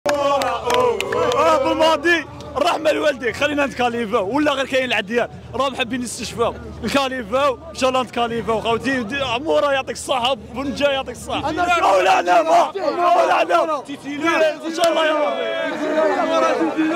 شاهد ثناء كبير من الجماهير الجزائرية على الزئبق محمد عمورة بعد أدائه اليوم في مباراة بوركينافاسو ❤
شاهد ثناء كبير من الجماهير الجزائرية على الزئبق محمد الأمين عمورة بعد أدائه اليوم في مباراة بوركينافاسو ❤